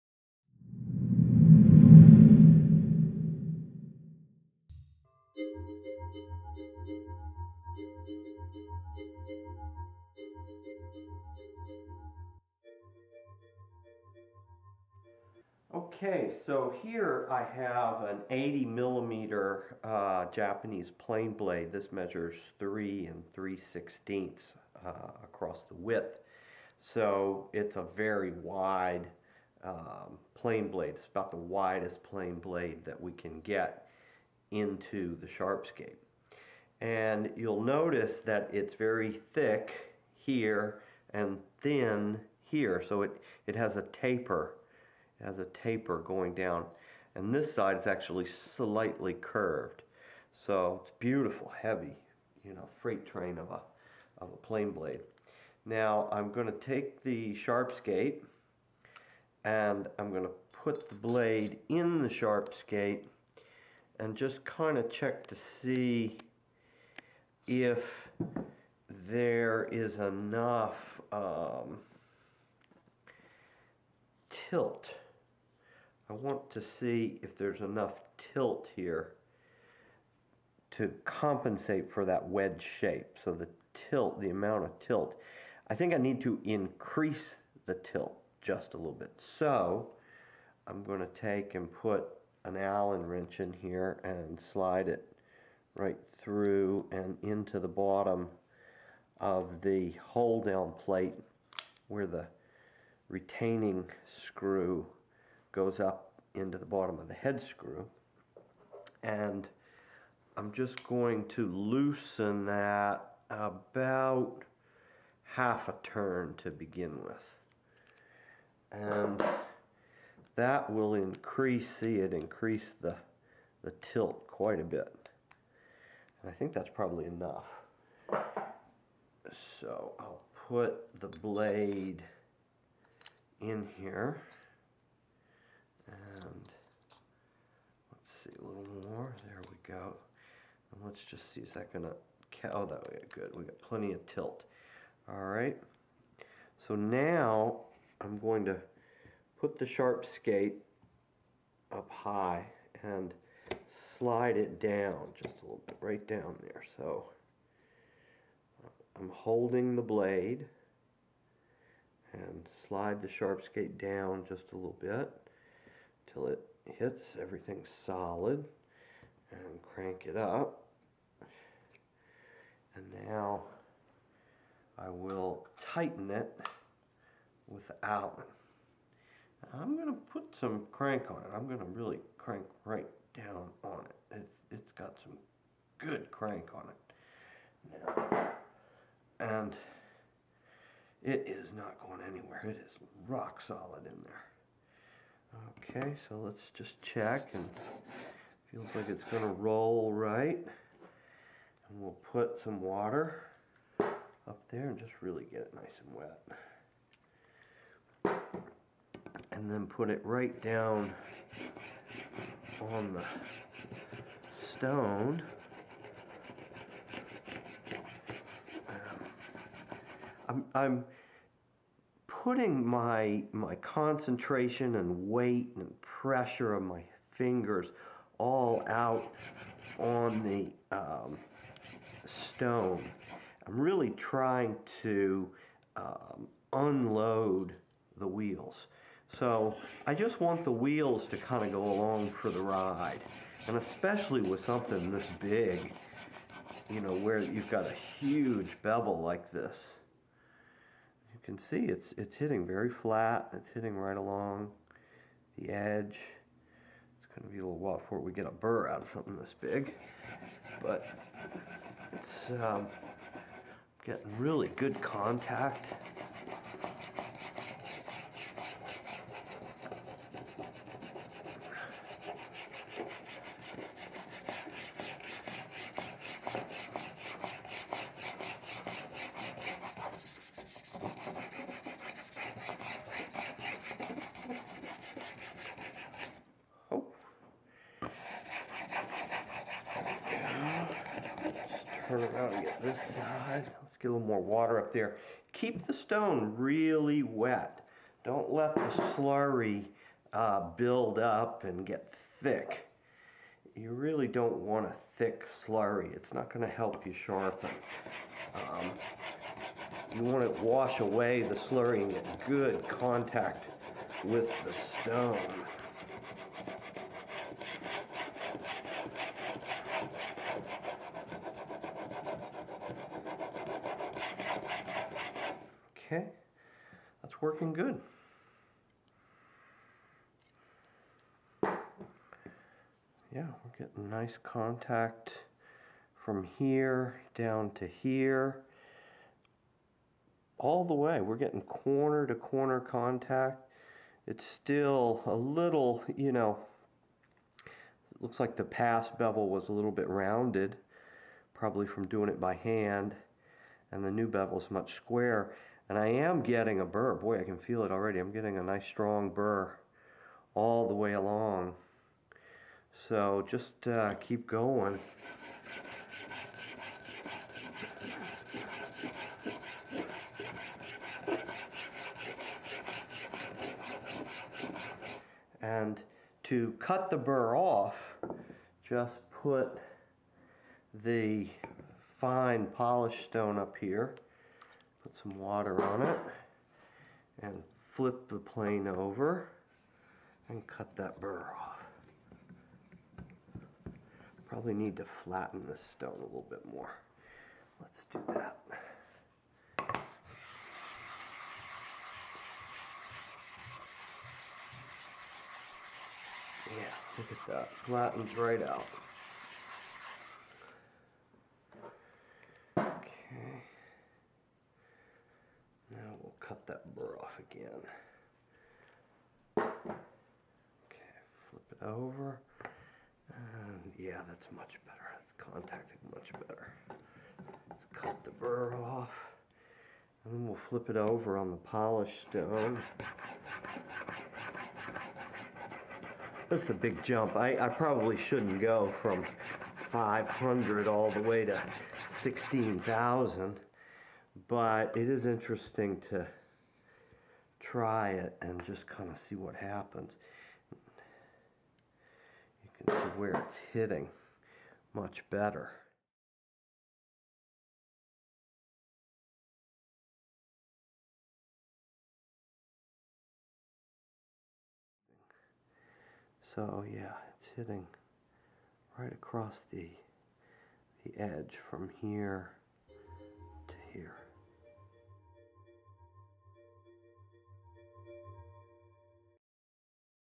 Sharpening a Japanese Plane Blade
Here Hap swiftly sharpens a Japanese plane blade, measuring 80mm and 3 3/16 of an inch.

(Note, in this video Hap uses a 500 grit stone, and a 16,000 grit stone. it is not recommended to make such a large jump in stones. However, one can still do such if they have the experience to do so.)